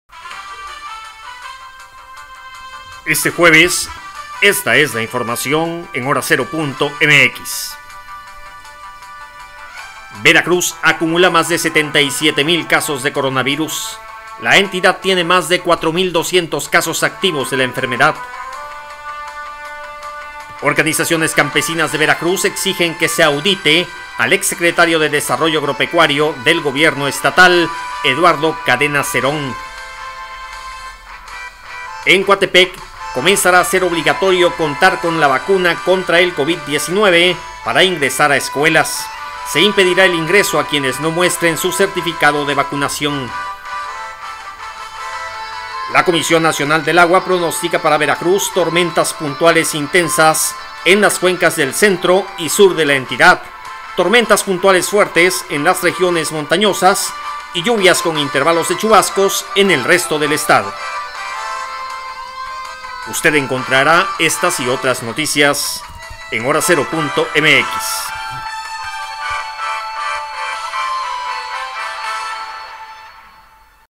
Resumen de noticias. Jueves 29 de julio de 2021
Este es el resumen de noticias de Hora cero Veracruz. Jueves 29 de julio de 2021